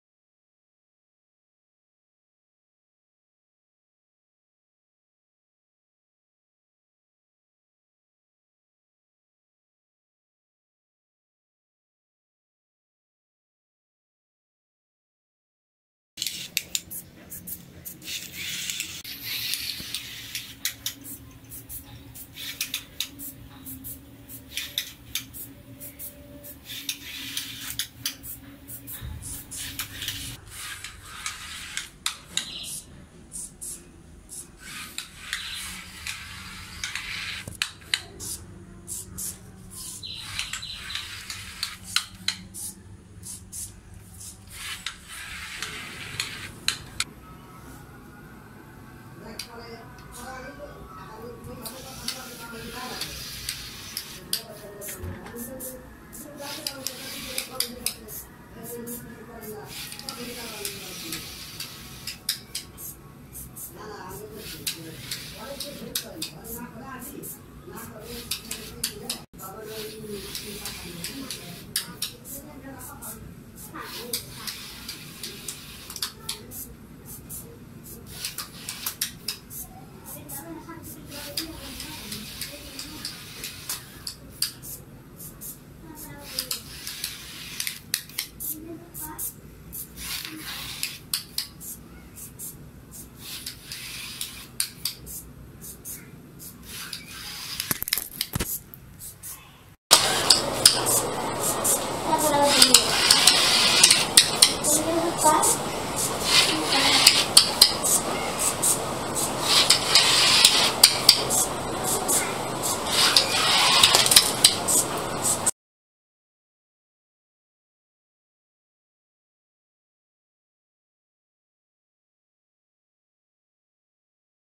Obstacle Avoiding Robot (simple car project)
Obstacle Avoiding Car (Test purpose)
(Arduino Code Only,, No circuit diagram available)